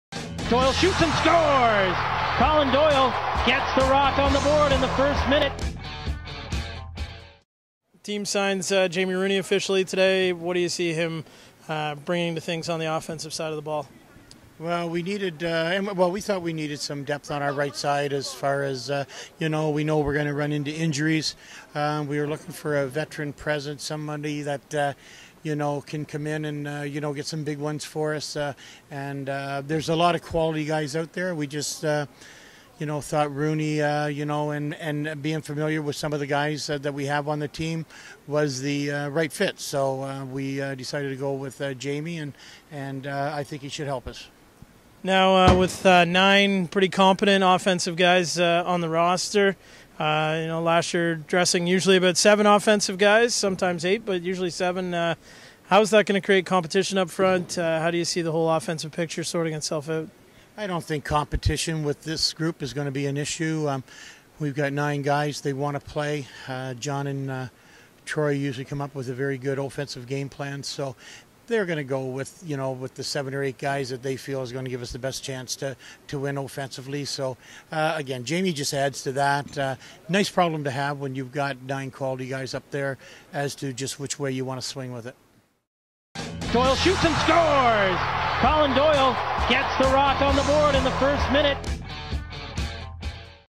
Toronto Rock Practice Air Canada Centre: Terry Sanderson
On Friday, December 30, 2011, the Toronto Rock had the opportunity to practice at Air Canada Centre in Toronto.  GM Terry Sanderson talked about the addition of Free Agent Jamie Rooney.